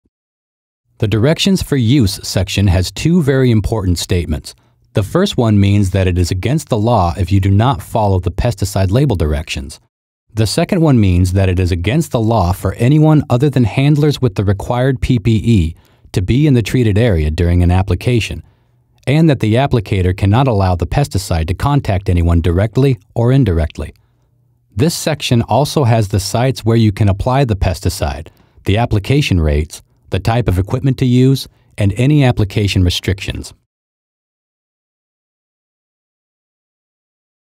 Discusses the Directions for Use box on a pesticide label
For Pesticide Handlers

This video alone does not meet the annual training requirements under the Worker Protection Standard (WPS). It can be used as part of a broader training program, which must be EPA-approved in its entirety. Handlers